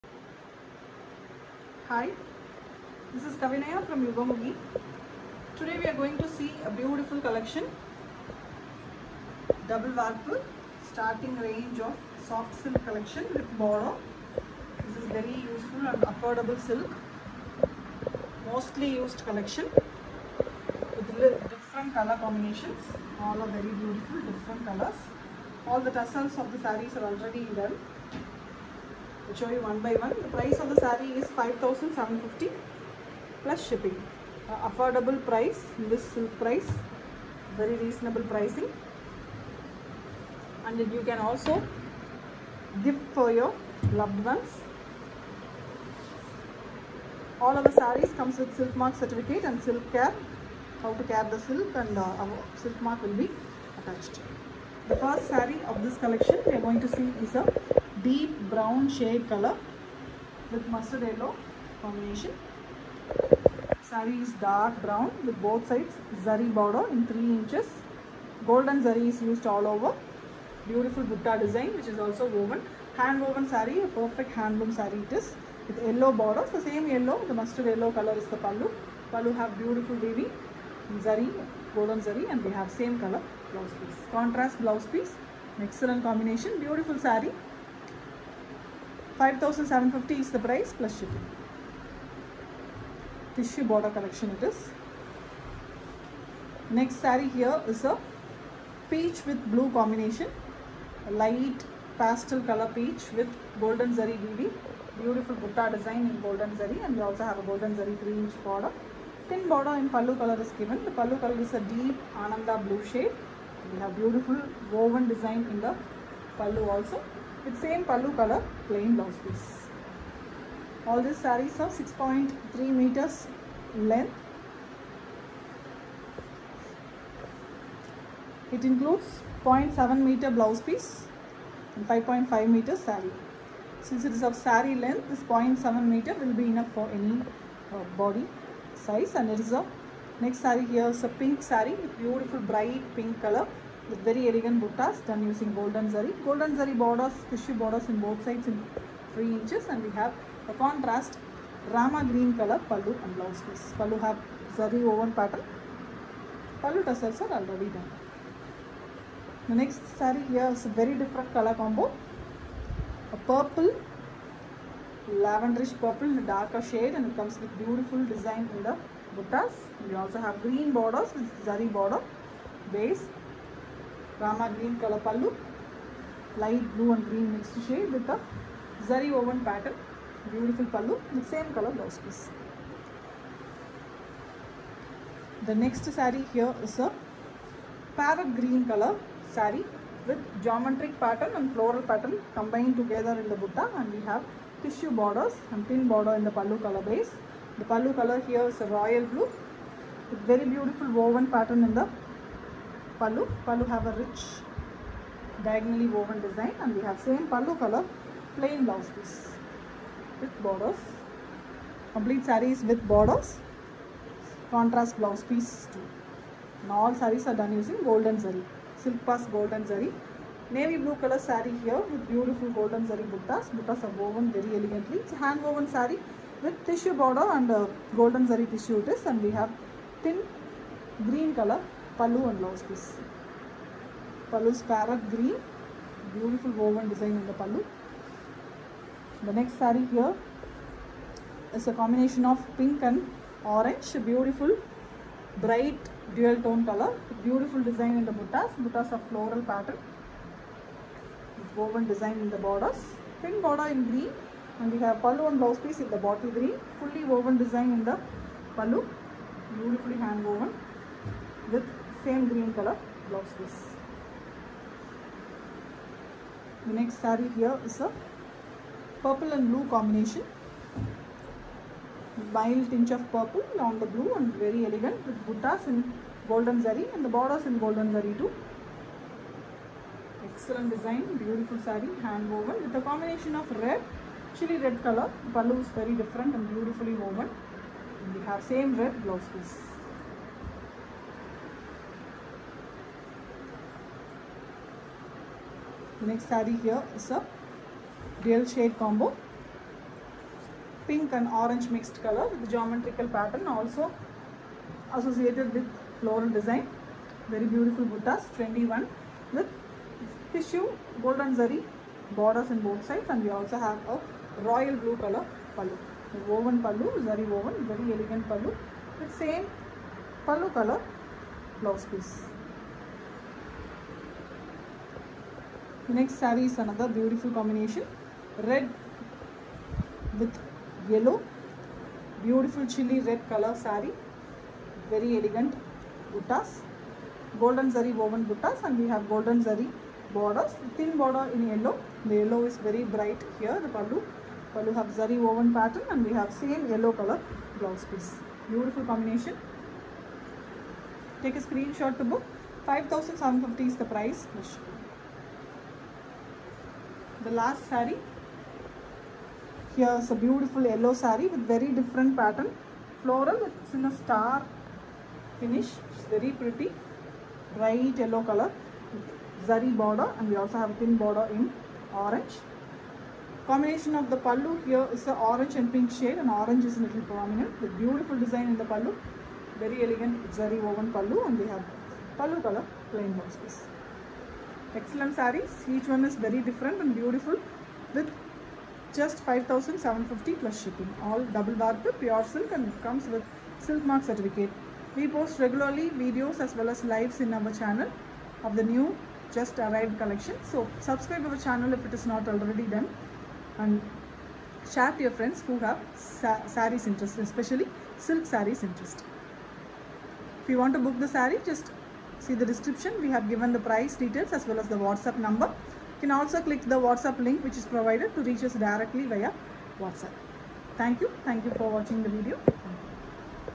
Pure kanchipuram soft silk sarees@5750 yugamugi 9789638283
Original Kanchipuram handloom Double vaarppu weaving exclusive design soft silk Sarees

Vibrant New colour shades

It’s 100% hand loom  weaving saree

2 days time required to weave this saree

Note : due to lighting effect color may vary slightly in appearance .only physical damage possible to return